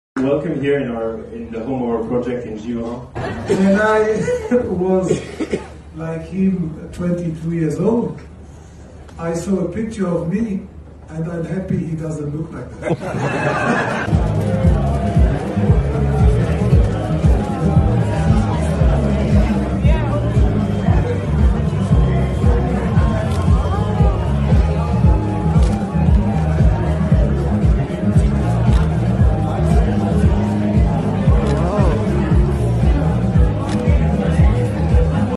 Montre BIVER. Lancement officiel 🔥 JCB
Jean-Claude Biver et son fils Pierre Biver nous parlent de leur nouvelle marque de montres BIVER